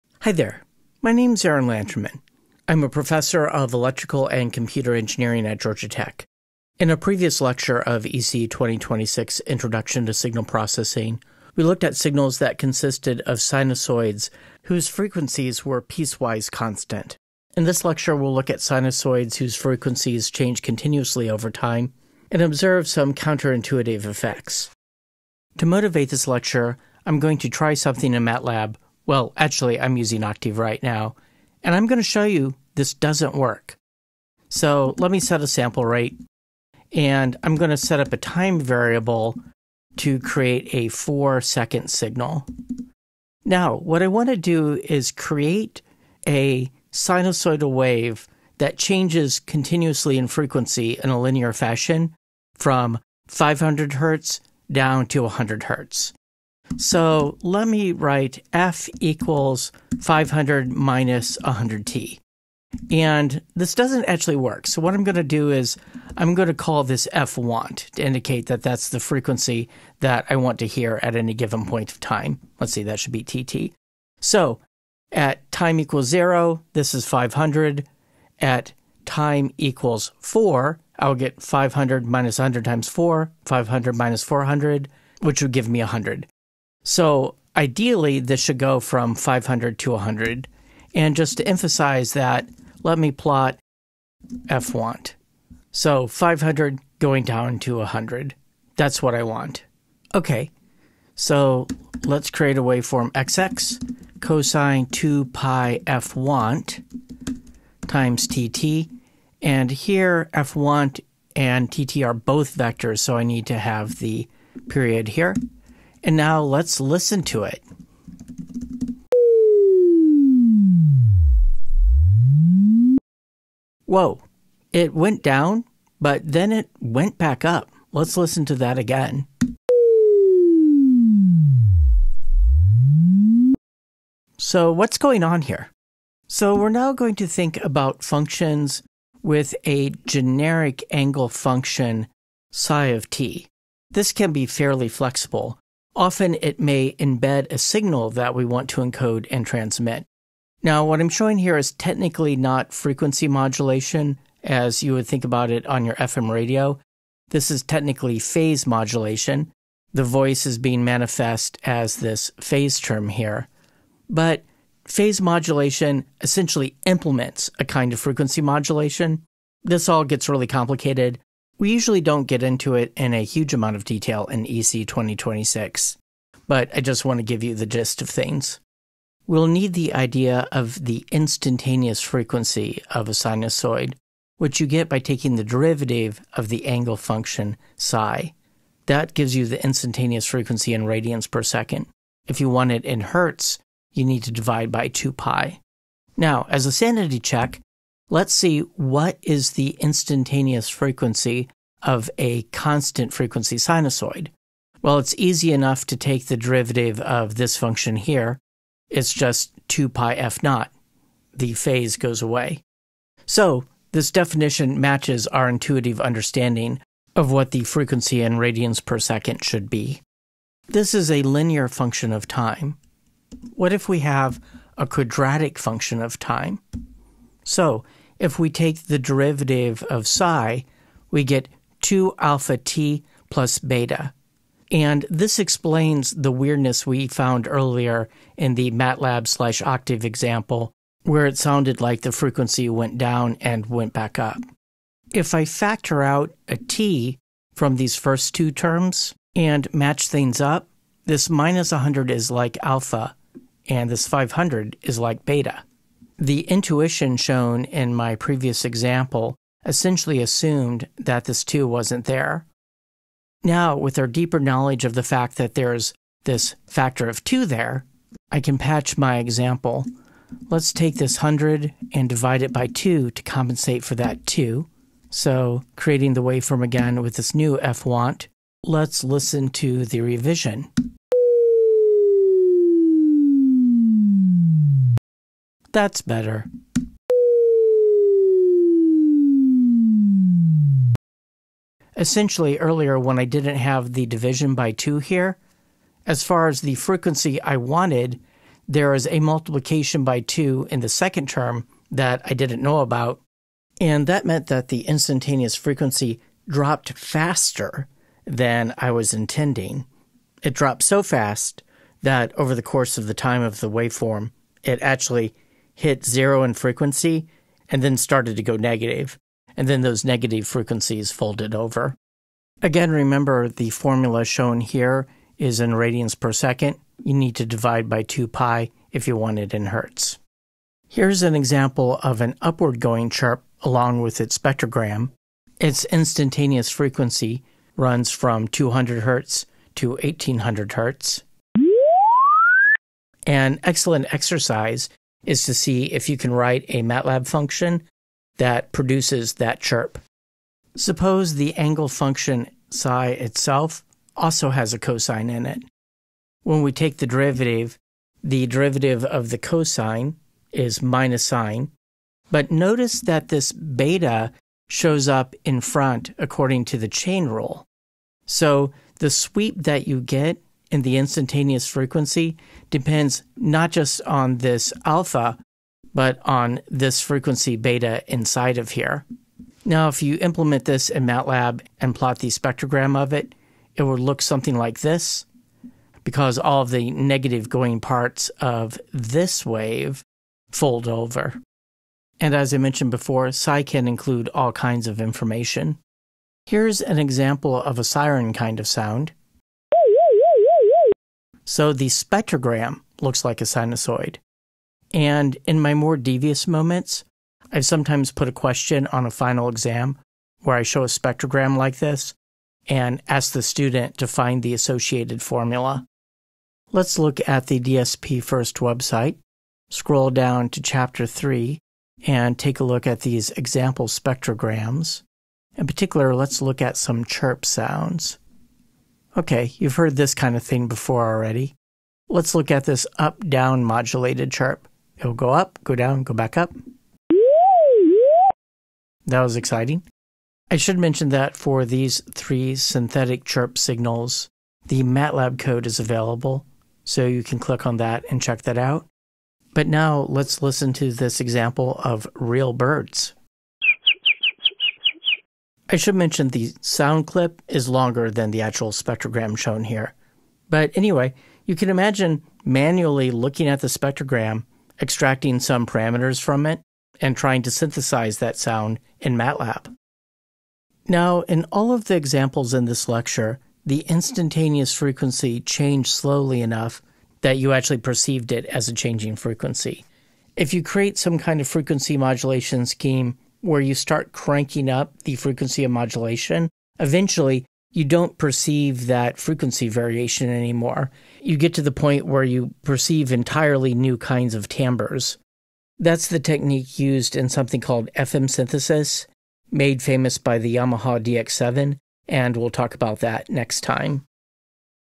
ECE2026 L12: Instantaneous Frequencies of Time-Varying Sinusoids (Introduction to Signal Processing)
0:00 Introduction
0:25 Counterintuitive example
2:16 Angle functions
2:59 Instantaneous frequency
3:40 Linear chirps
4:16 Fixing earlier example
5:30 Upward chirp
5:50 Sinusoidal modulation
6:56 Website examples
7:29 Bird sounds
7:50 FM synthesis preview